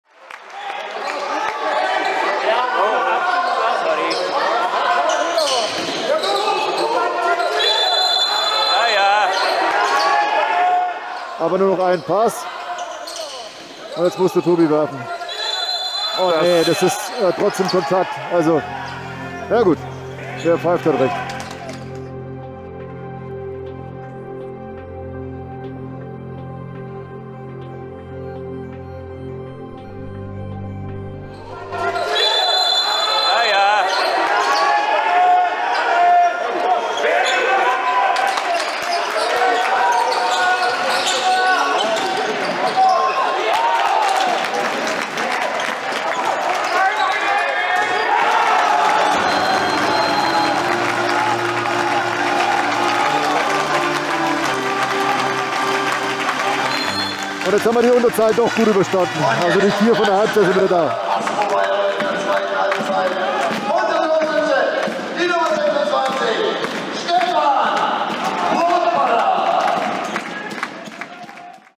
Handballregeln: Schritte – und schaut euch mal die Zeitlupe genau an …
… ich hab erst da gesehen, daß SchwarzNr.6 gewaltig am Trikot des Gegners zieht .